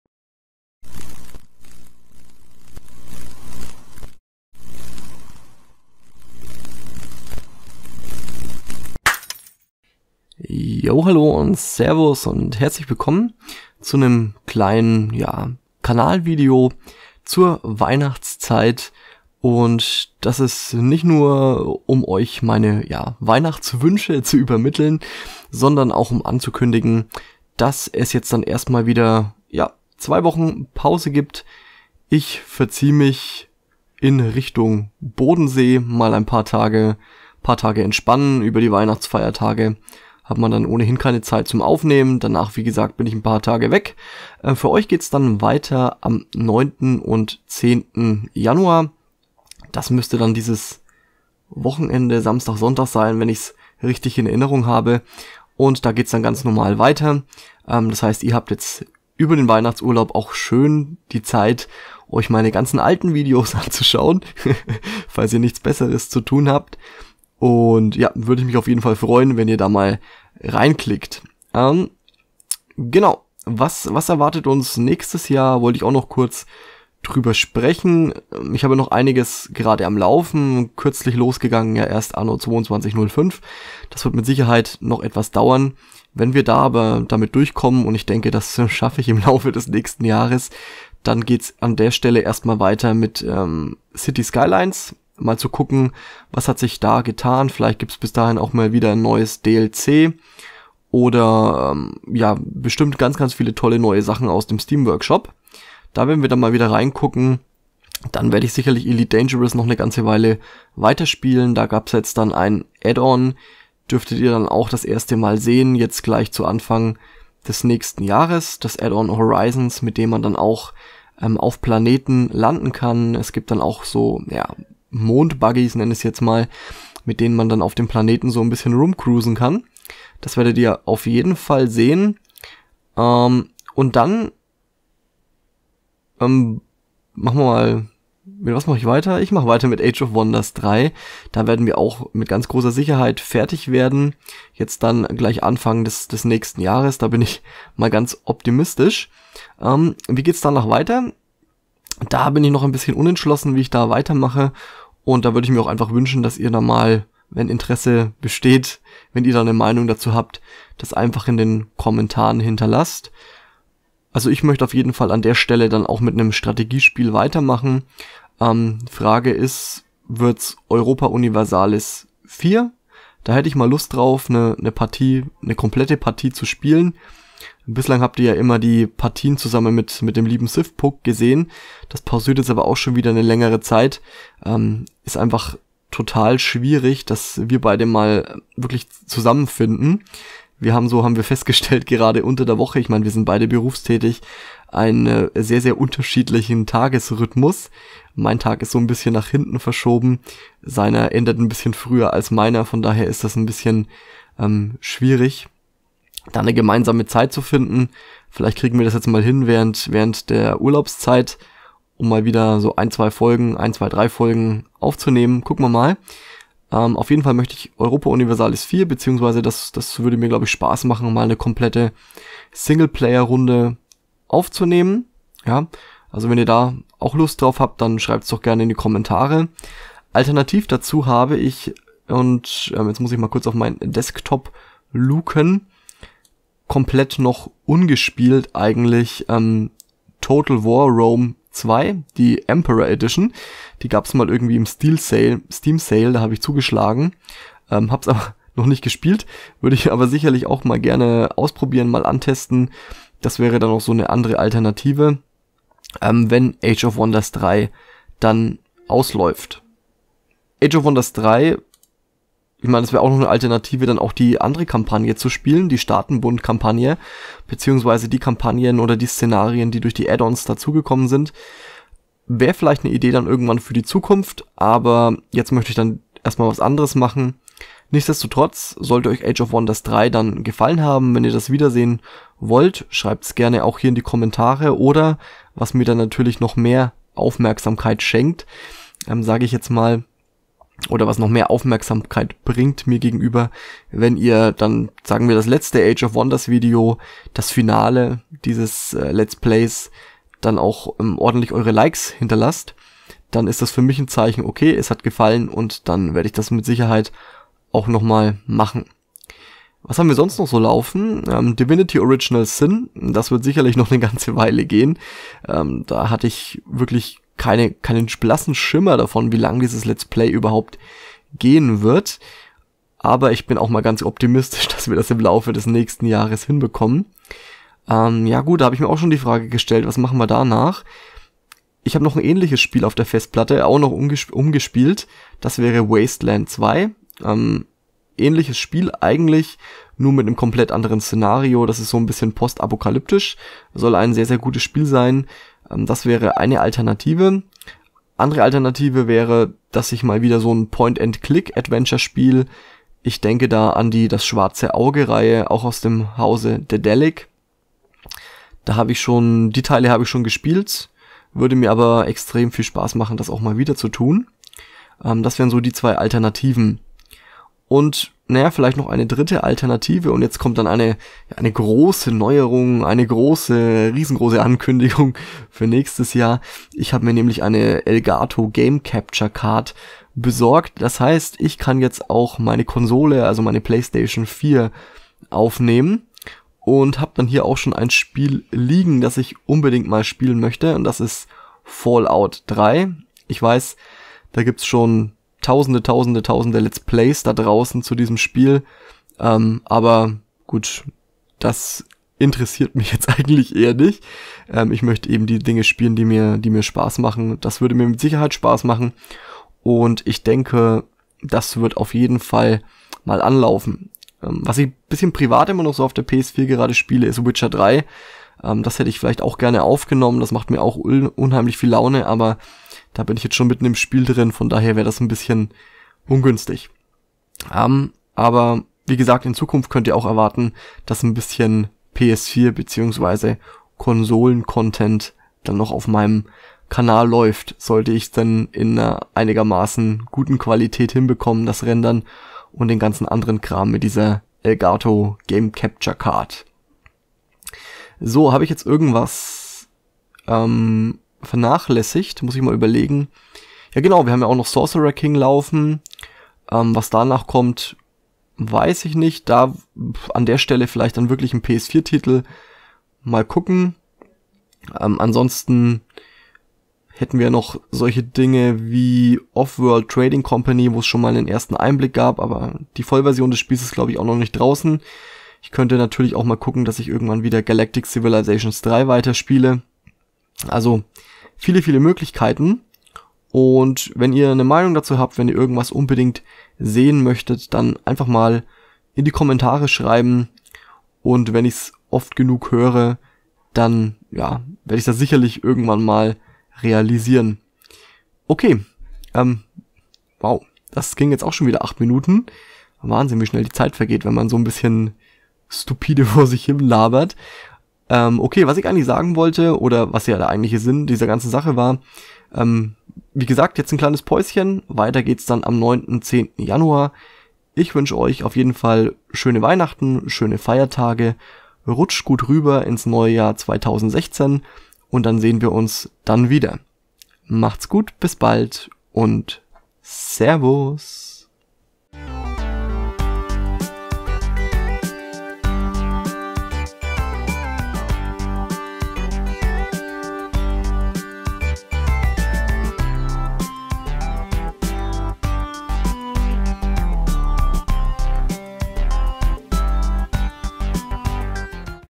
Weihnachtspause 2015/16
Ich wünsche all meinen Zuschauern frohe Weihnachten und einen guten Rutsch ins Jahr 2016.

Auch ich gehe in die Weihnachtspause. Weiter geht's am 9. Januar 2016.